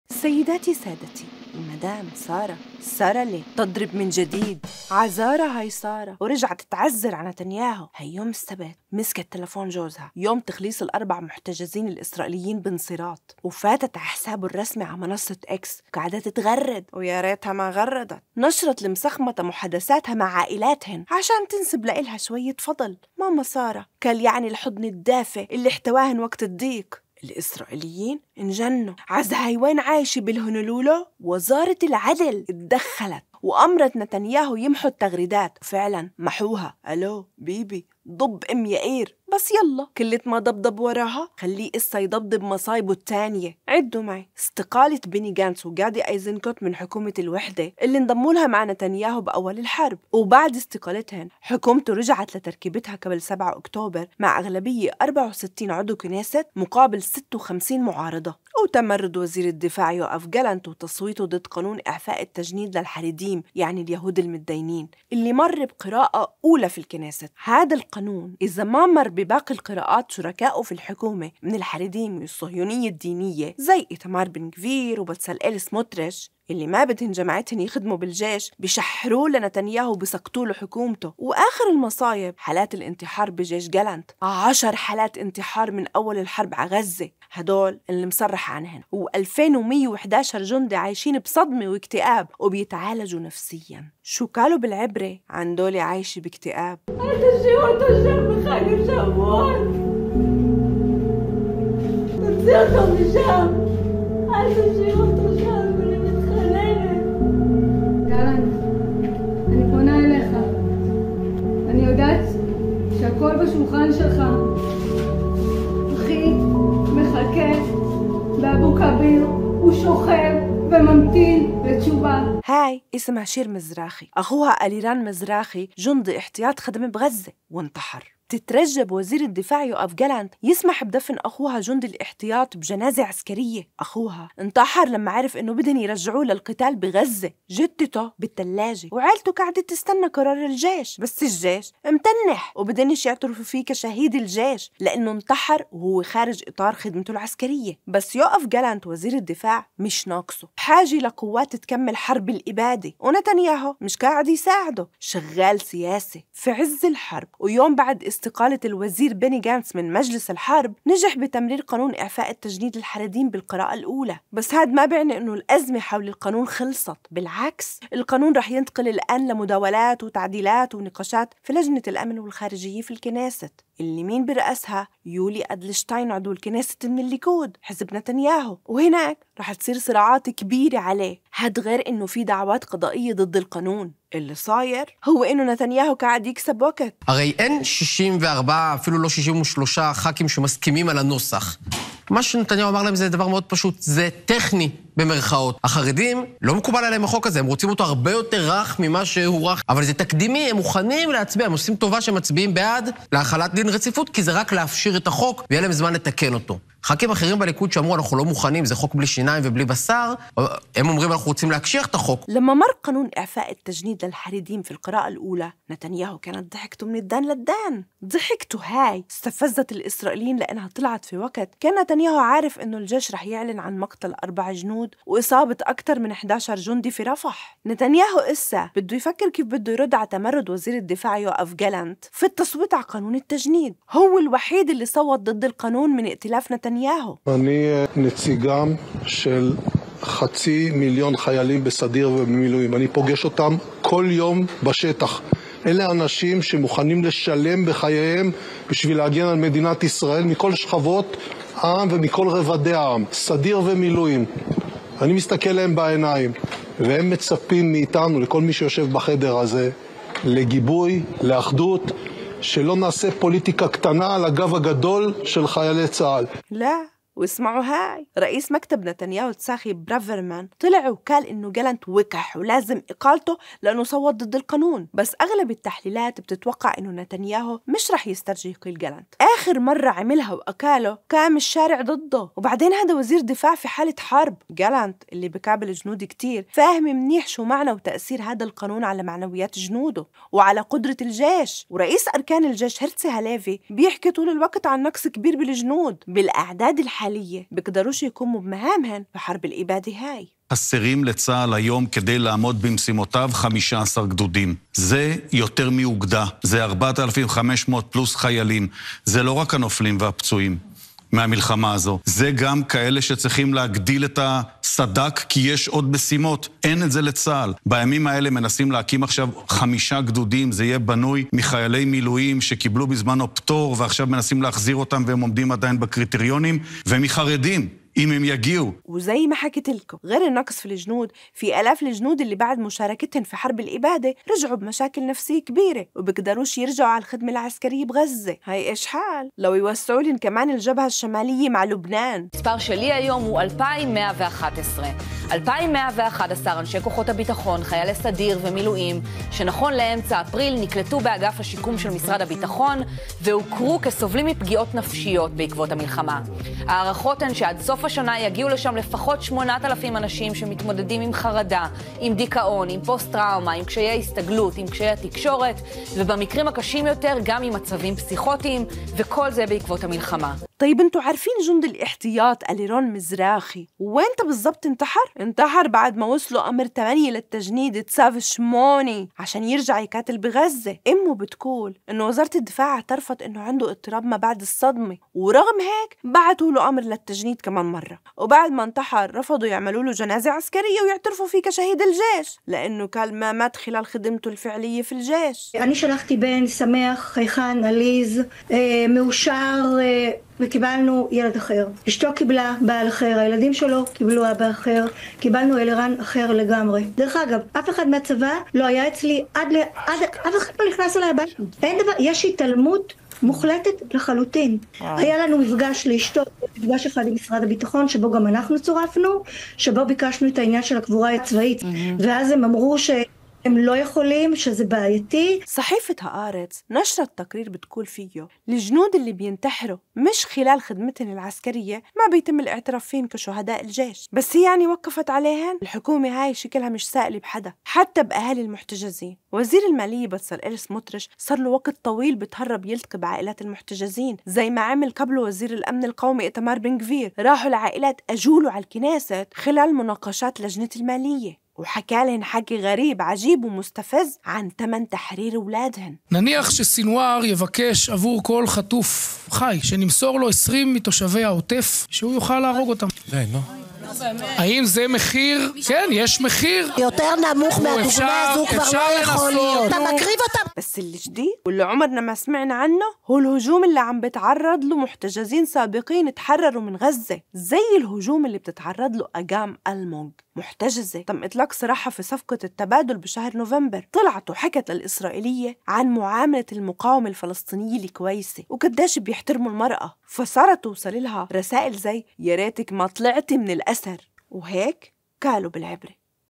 شو قالوا بالعبري | انتحار وأزمات نفسية بصفوف جيش الاحتلال!
انتحار وأزمات نفسية بصفوف جيش الاحتلال!